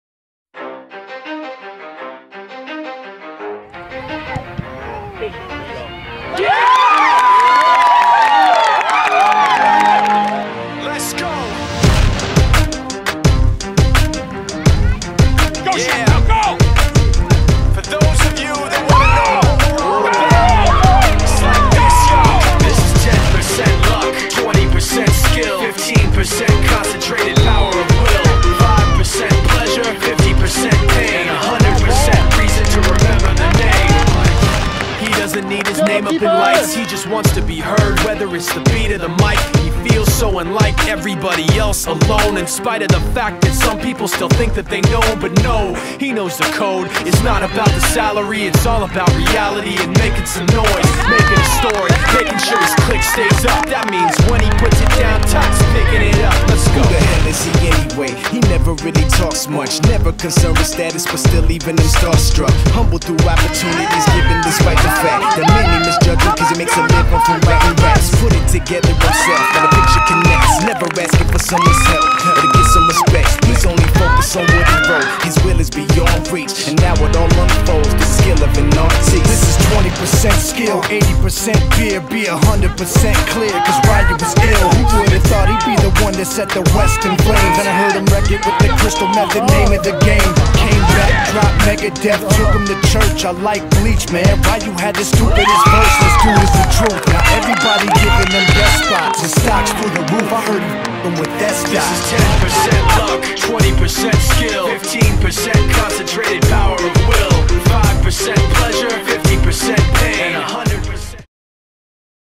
Santiago High Soccer - NOCC
A dramatic win for the Santiago girls Frosh/Soph team against a very good El Toro team.  This win put them into the Finals.